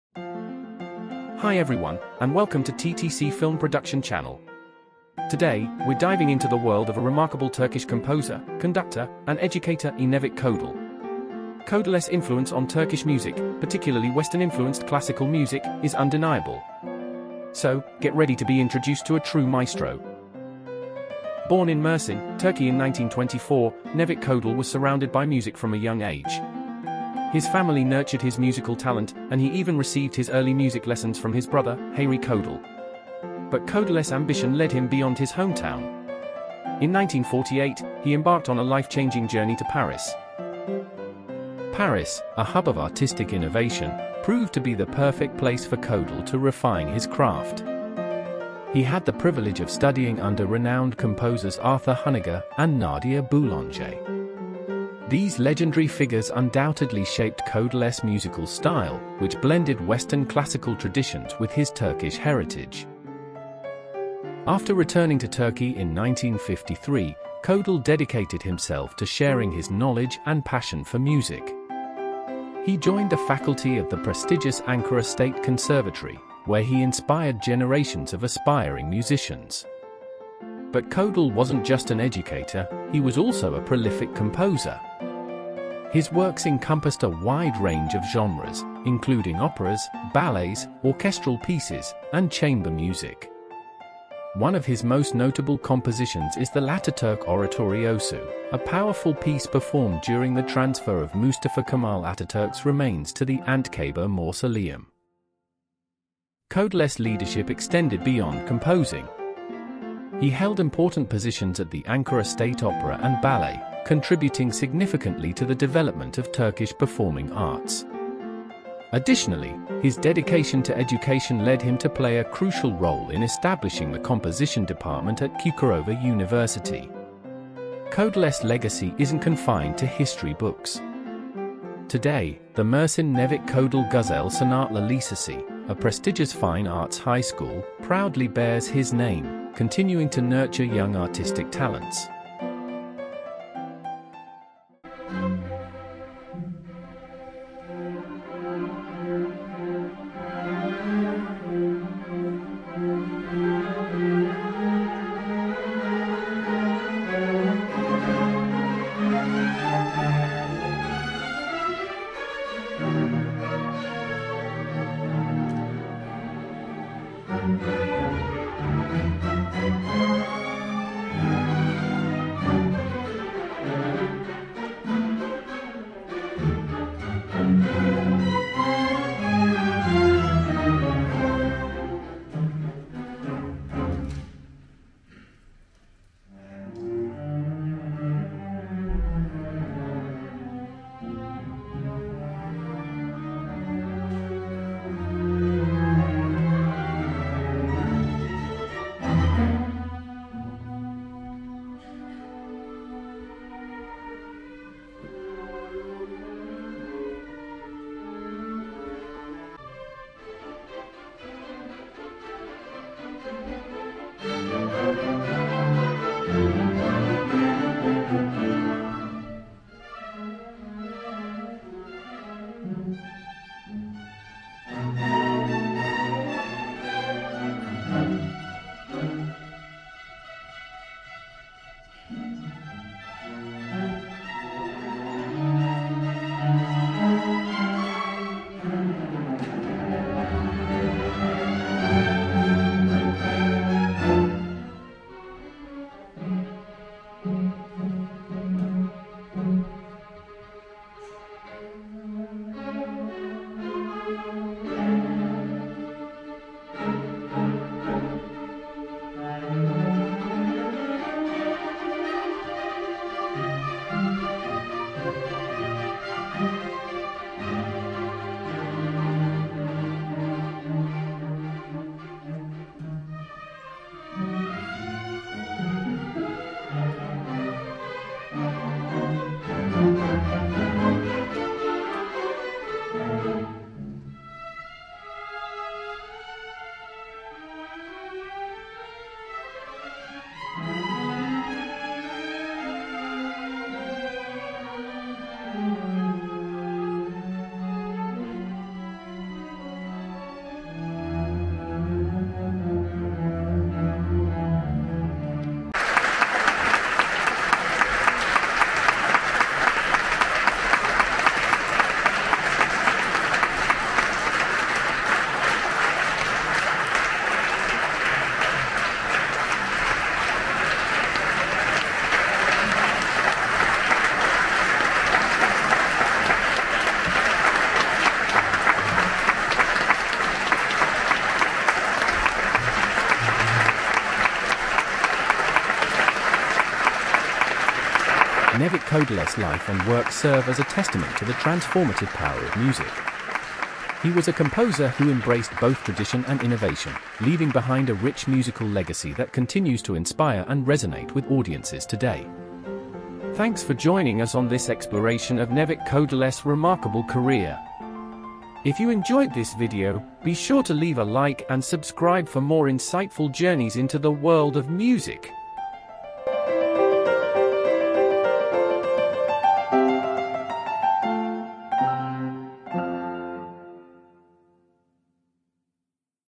🎹 Nevid Kodallı: A Musical Maestro's Legacy | Classical Music
concert nevidkodallı classicalmusic Nevid Kodallı stands as a towering figure in the realm of music, his name synonymous with innovation, creativity, and profound artistry. Born in Turkey, Nevid Kodallı's journey into the world of music began at a young age, where his innate talent and passion for composition became evident.

Throughout his illustrious career, Nevid Kodallı left an indelible mark on the musical landscape, captivating audiences with his mesmerizing compositions and virtuoso performances.

As a composer, Nevid Kodallı crafted symphonies that transcended boundaries, seamlessly blending traditional Turkish melodies with modern harmonies, thereby creating a distinctive musical language that resonates with listeners worldwide.

His symphonic works, characterized by their emotive depth and evocative storytelling, continue to enrapture audiences and inspire generations of aspiring musicians.

Beyond his prowess as a composer, Nevid Kodallı's contributions as a conductor are equally remarkable. With his unparalleled interpretative skills and commanding presence on the podium, he brought to life some of the most iconic orchestral works, infusing each performance with his unique artistic vision and unwavering dedication to musical excellence.

Nevid Kodallı's conducting style, characterized by its precision and passion, earned him widespread acclaim and established him as a leading figure in the classical music scene.

Throughout his career, Nevit Kodallı remained committed to nurturing young talent and fostering a deep appreciation for music among audiences of all ages. His legacy extends far beyond his own compositions and performances, encompassing his role as an educator and mentor to countless aspiring musicians. Through his teachings and mentorship, Nevit Kodallı imparted invaluable knowledge and insight, shaping the next generation of musical virtuosos and ensuring that his influence would endure for years to come.

Today, Nevid Kodallı's legacy continues to thrive, his name revered by musicians and music enthusiasts around the world. Through platforms like YouTube, his timeless compositions and performances reach new audiences, allowing his musical genius to transcend temporal and geographical boundaries.

As we delve into the vast repertoire of Nevid Kodallı's works, we embark on a journey of discovery and enlightenment, guided by the transformative power of music and the enduring legacy of a true musical maestro. Join us as we celebrate the life, works, and legacy of Nevid Kodallı, a beacon of inspiration in the world of music.

All material in this video is original and personally recorded on-site by TTC Film Production.
TTC Film Production 2024